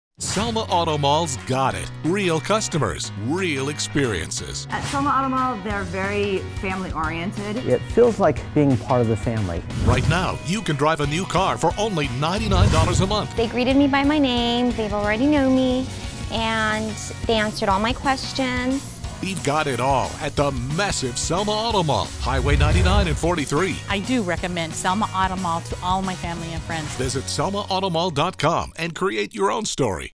Selma Auto Mall treats customers like family
Hear from customers how Selma Auto Mall treats their customers like family.